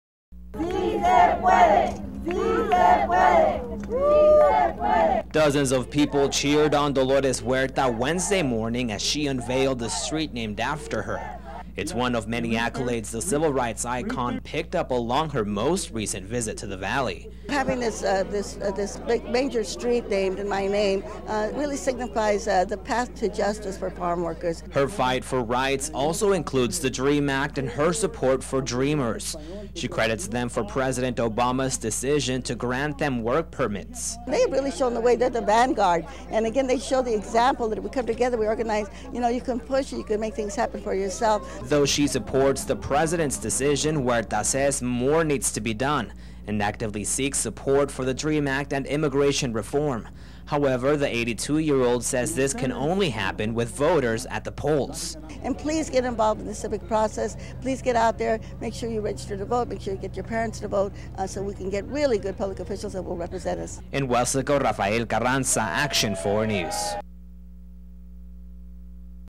Huerta Honored for Work, Pushes for DREAM Act
Dozens of people cheered on Dolores Huerta Wednesday morning as she unveiled the street named after her. It's one of many accolades the civil rights icon picked up along her most recent visit to the valley. Her fight for rights also includes the Dream Act and her support for dreamers. She credits them for President Obama's decision to grant them work permits.